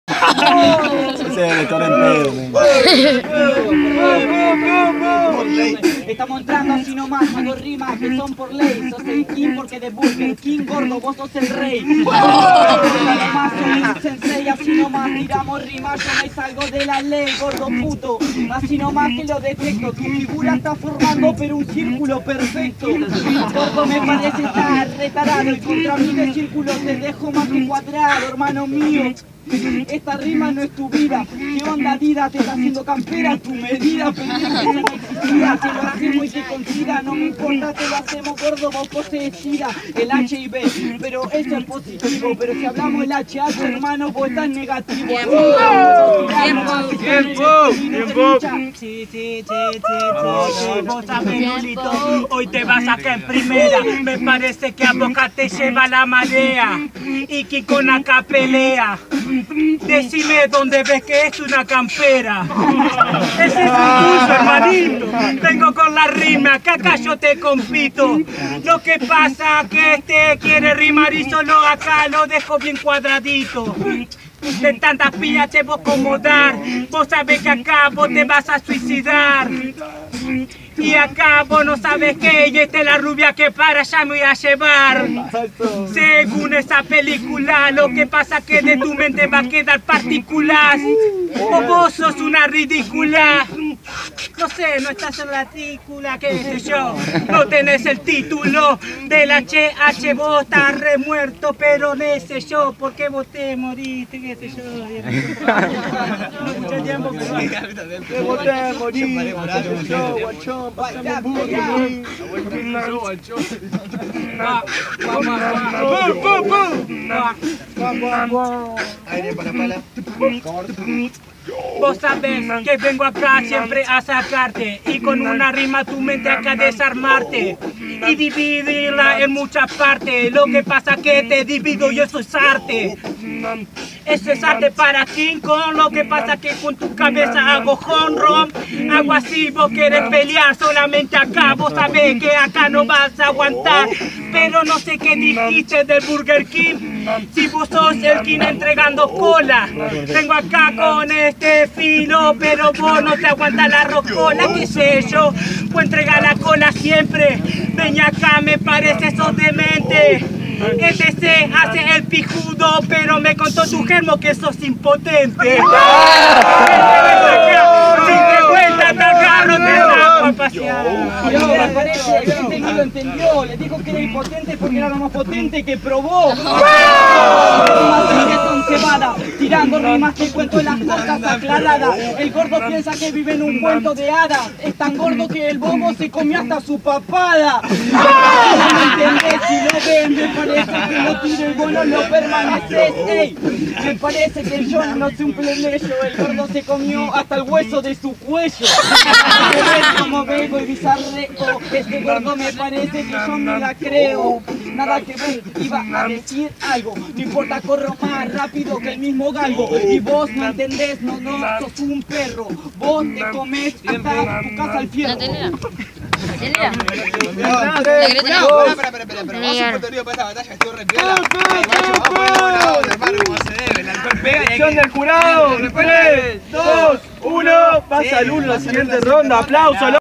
Lulo vs King Kong 8vo Fridaay Freestyle 2da Temporada
Modalidad: 1 vs 1

Lugar:Santa Fe,Santo Tomé costanera(anfiteatro)

Ganador: Lulo